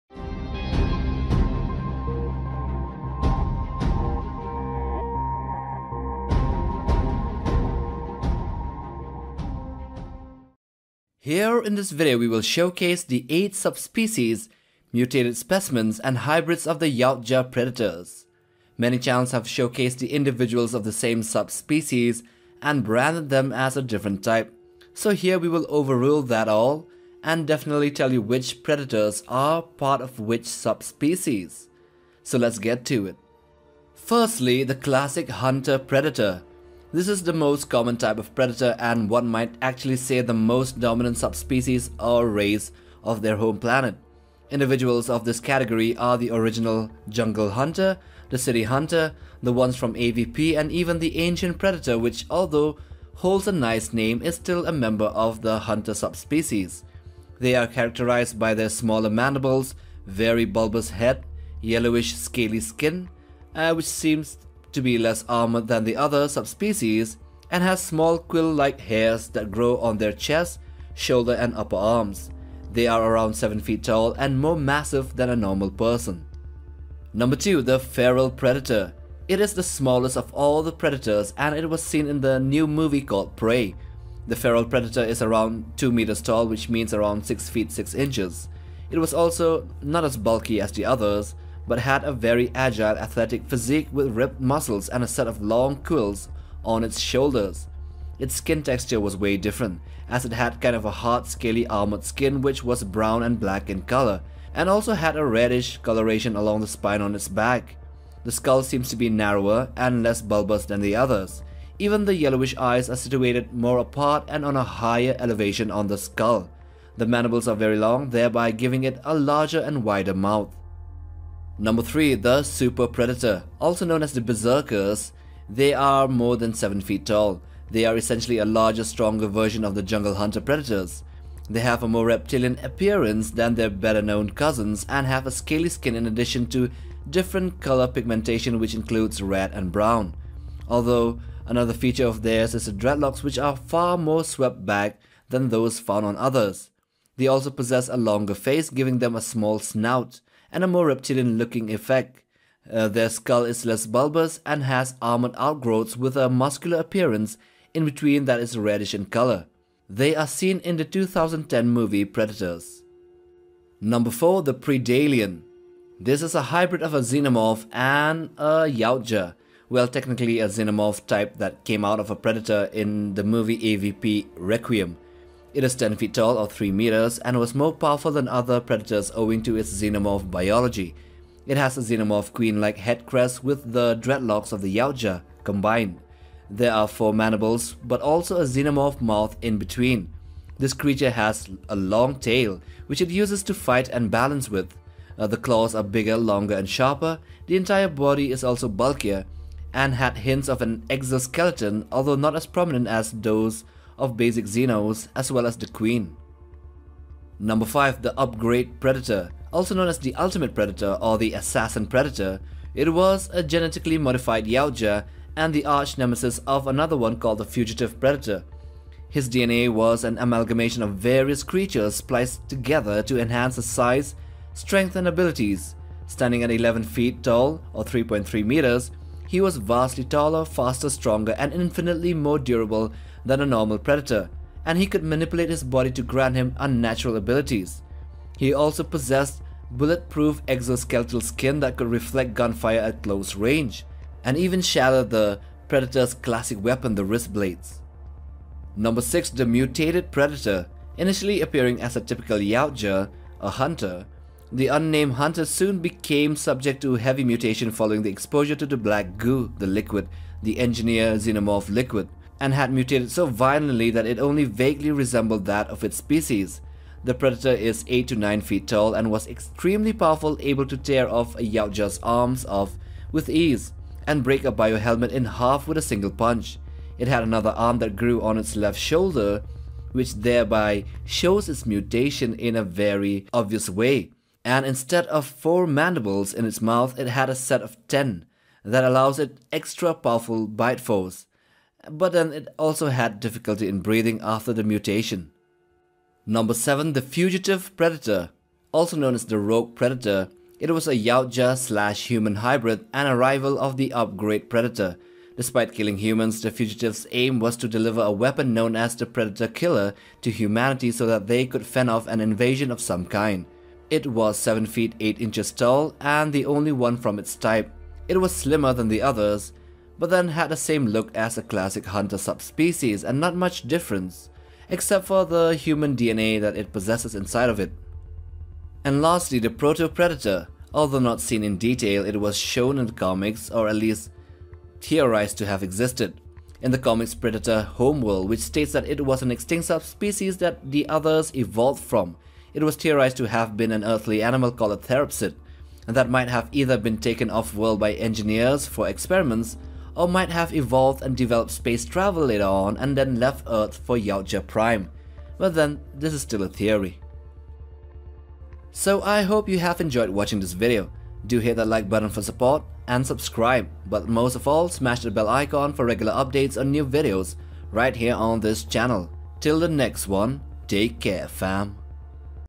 The 8 Predator Subspecies And Hybrids Explained
The Predator is the titular extraterrestrial species featured in the Predator science fiction franchise, characterized by its trophy hunting of other species for sport. There are atleast 8 different subspecies and hybrids of Yautja/Predator that we know of by far. So, in this video, we briefly explain to you about each of them.